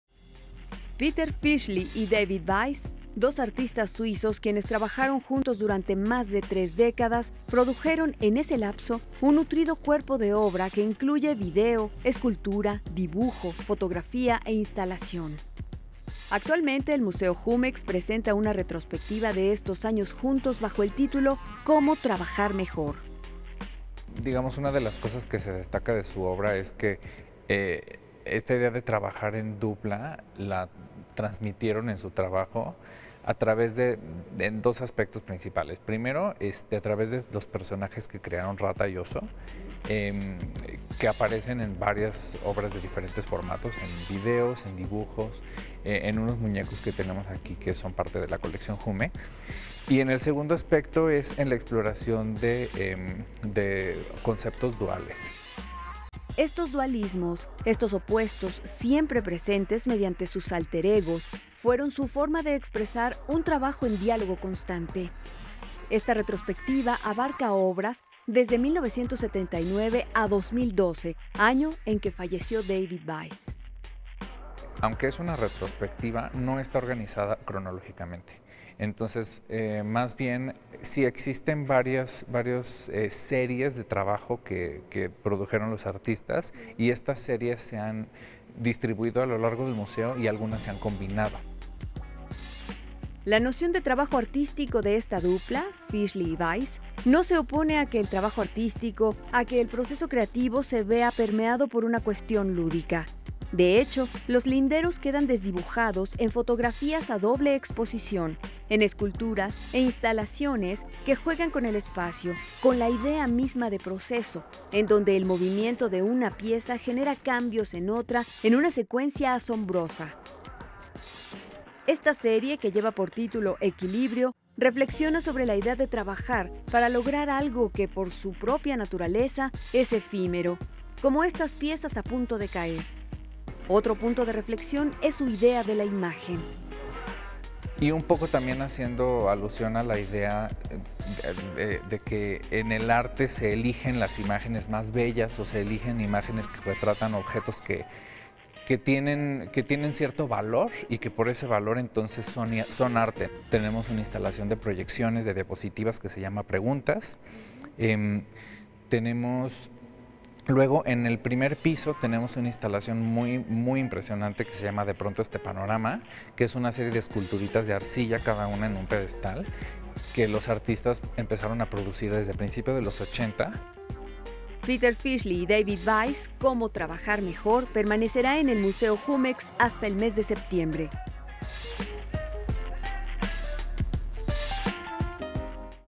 Fischli y Weiss llegan al Museo Júmex
Peter Fischli y David Weiss, dos artistas suizos quienes trabajaron juntos durante más de tres décadas, produjeron en ese lapso un nutrido cuerpo de obra que incluye video, escultura, dibujo, fotografía e instalación.  Actualmente, el Museo Júmex presenta una retrospectiva de estos años juntos bajo el titulo "¿Cómo trabajar mejor?".
Por: Alejandra Flores.